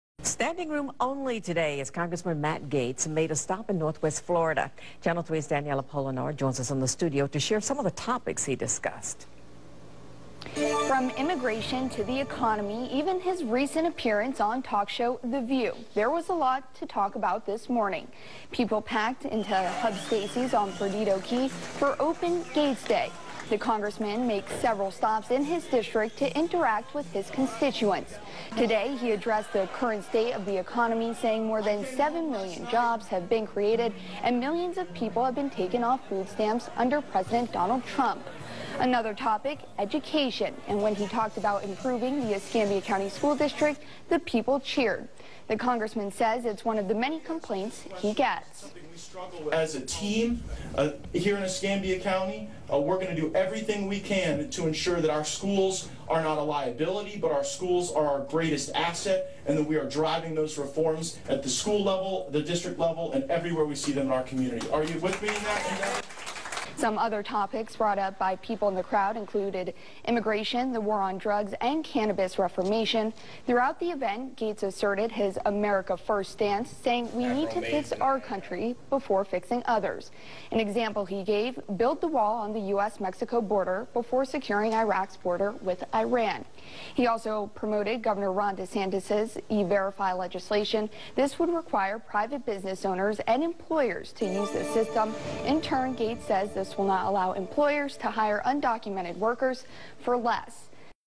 Open Gaetz Day Was a Success!
Thanks to everyone who joined us for OpenGaetz Day yesterday in Perdido Key, Destin and Laurel Hill!